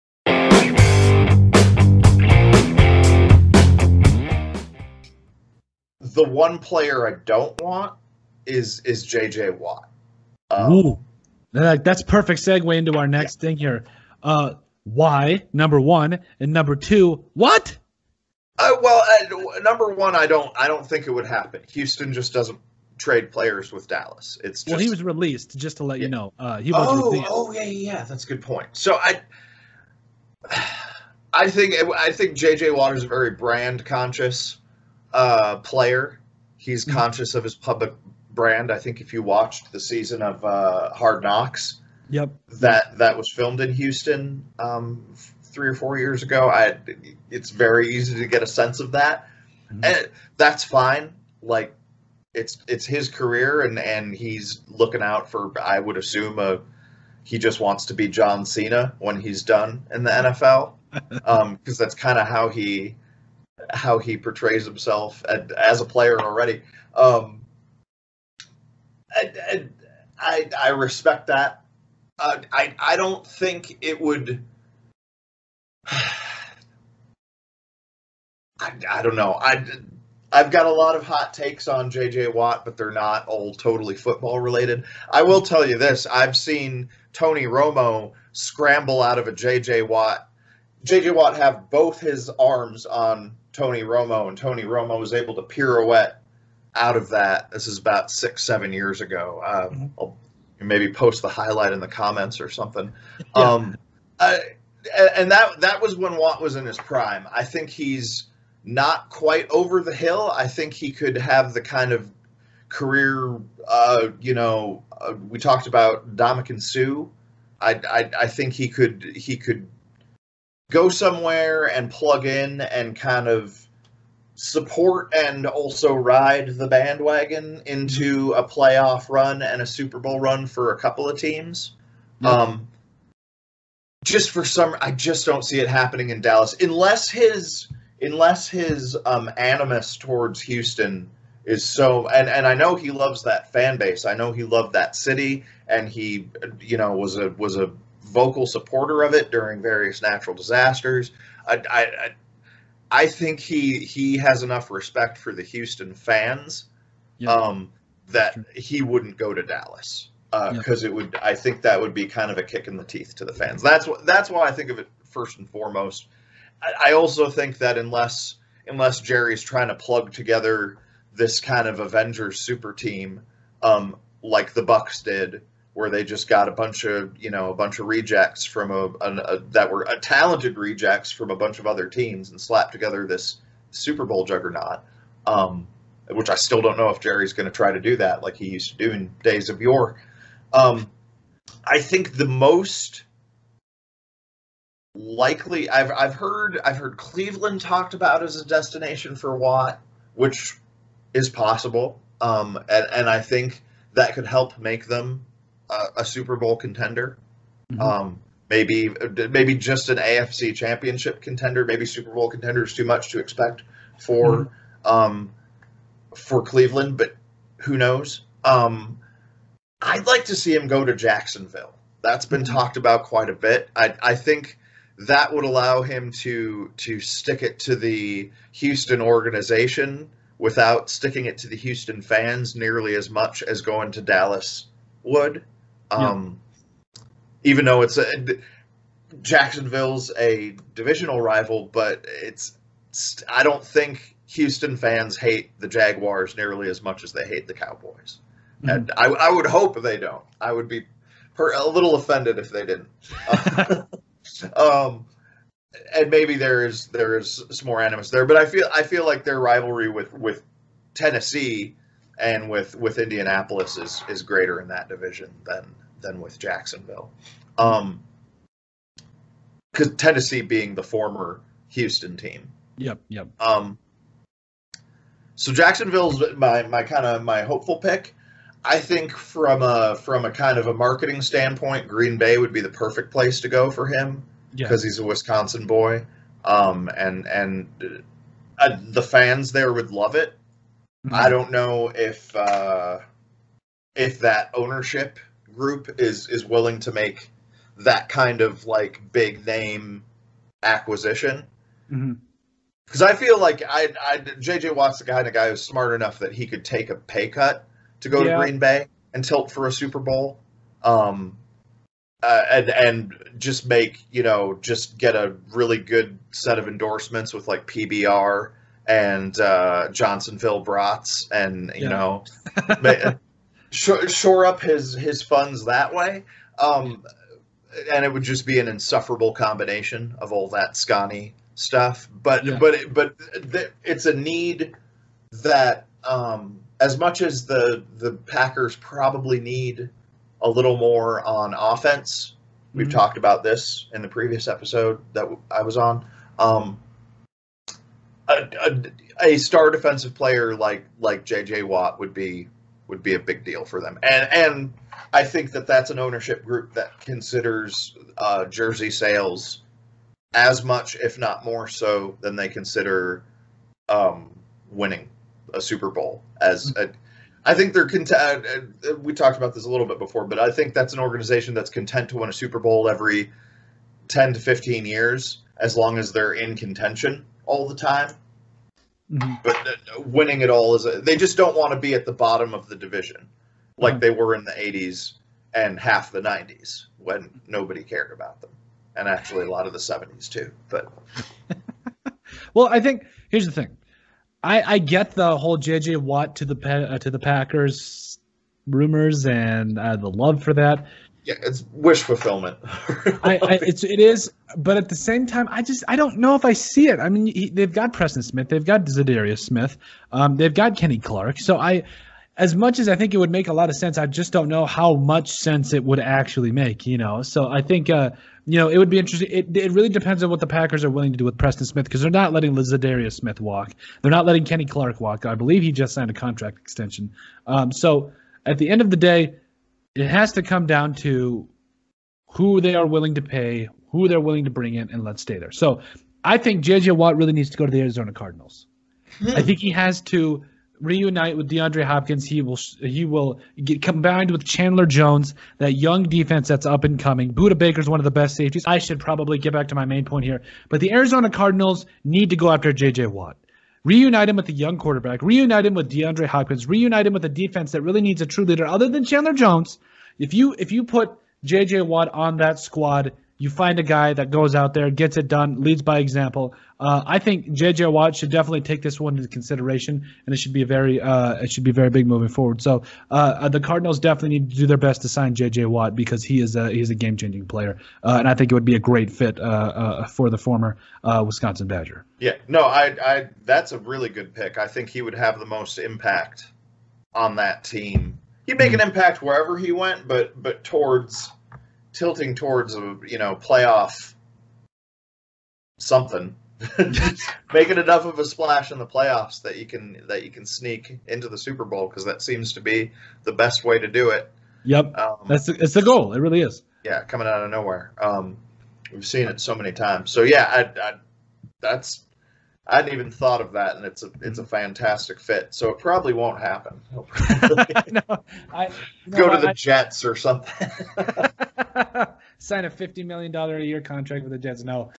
The Cardinals NEED TO GO AFTER J.J. WATT | The Sports Brief Podcast
After J.J. Watt was released from the Houston Texans, many teams were in play for the former Wisconsin Badger.

The Arizona Cardinals should be ALL IN on Watt. In fact, they should GO AFTER HIM in free agency.